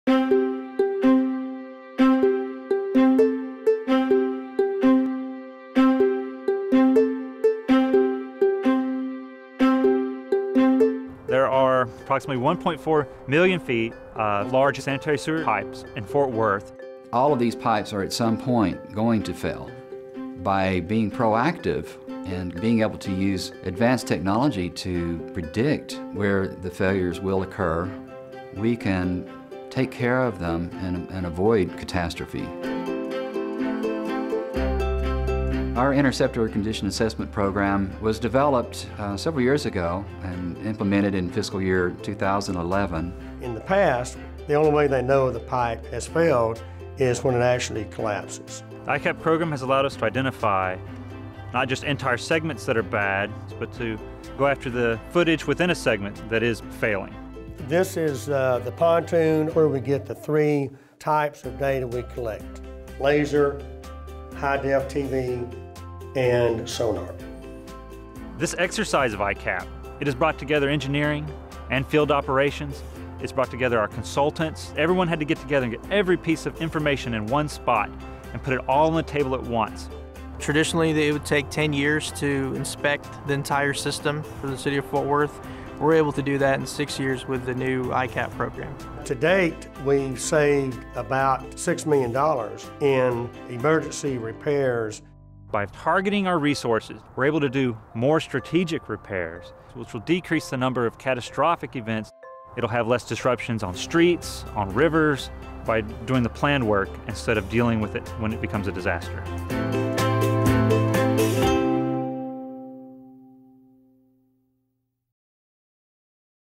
2016 TEEA Winner Innovative Operations: City of Fort Worth Water Department
Beneath the streets and along the Trinity River that weaves through Fort Worth lies a 262-mile matrix of large-diameter sanitary-sewer pipes—“interceptors”—feeding into a single wastewater treatment plant that serves the growing metropolis. The monitoring and maintenance of this critical infrastructure falls under the responsibility of the Fort Worth Water Department and the contractors it relies on to inspect, maintain, repair, and plan for replacement, to keep water flowing across the city.